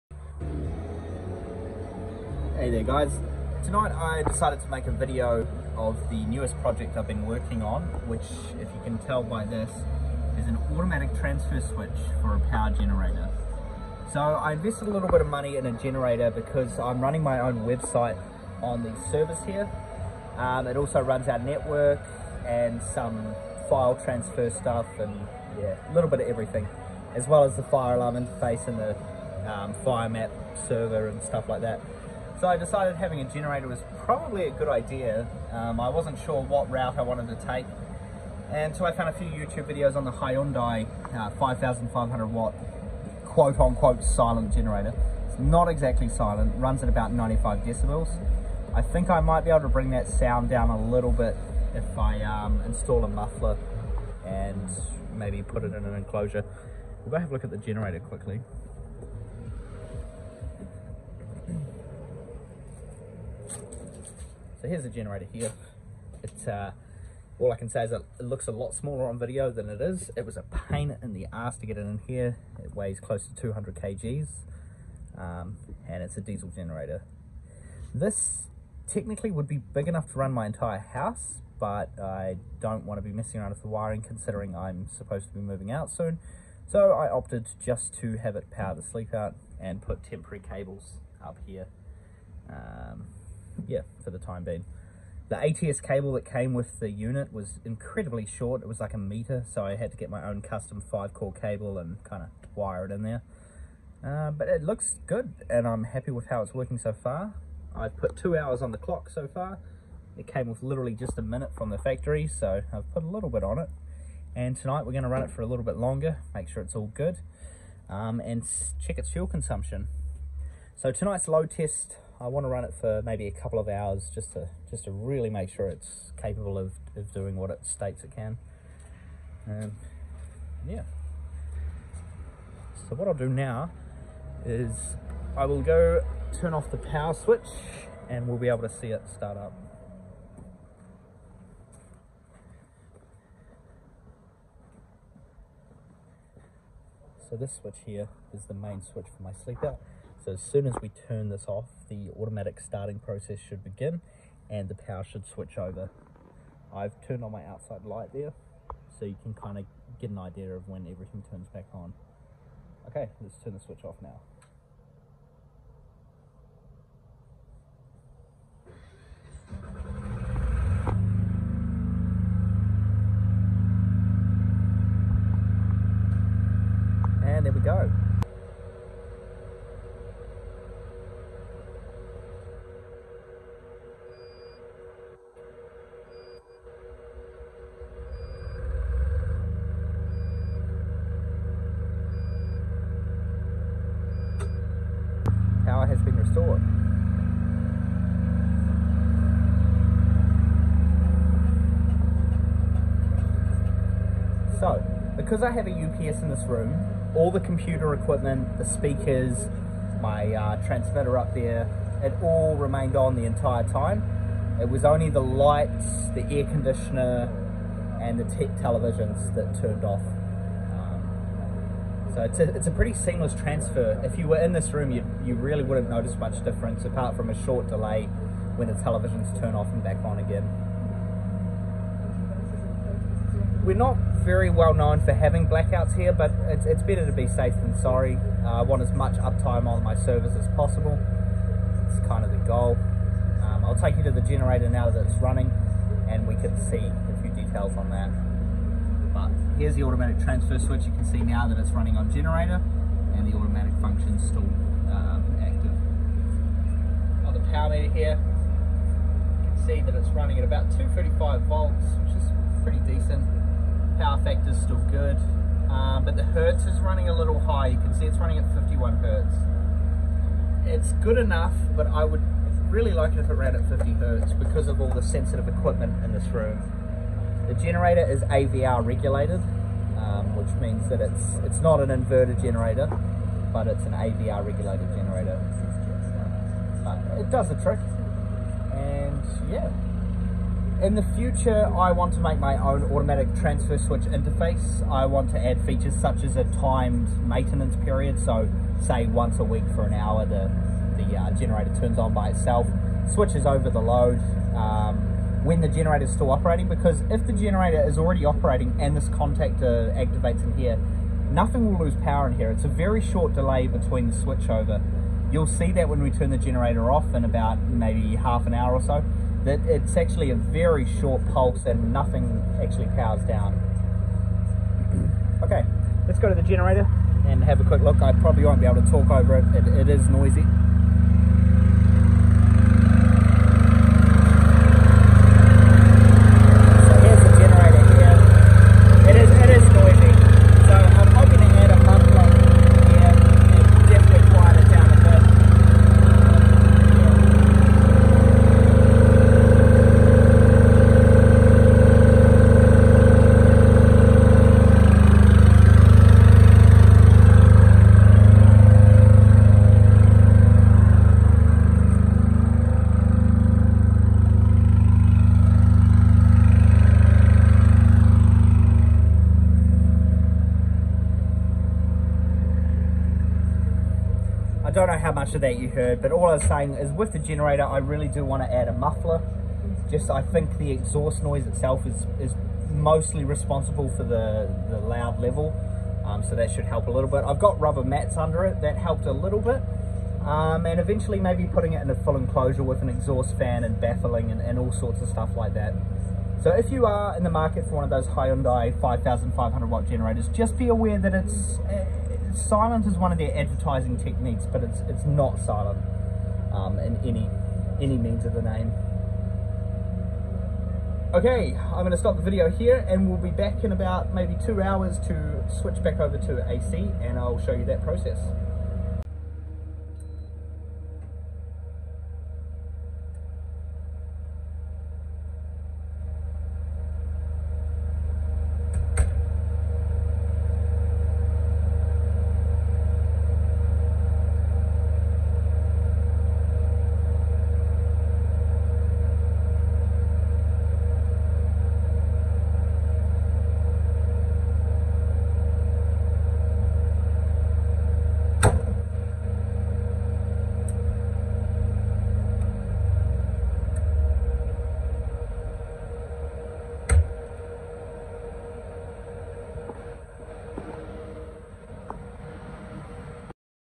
Back up Generator project
This video showcases my newly installed home standby generator. I have been wanting to do this project for many years but didn't want to rush into it.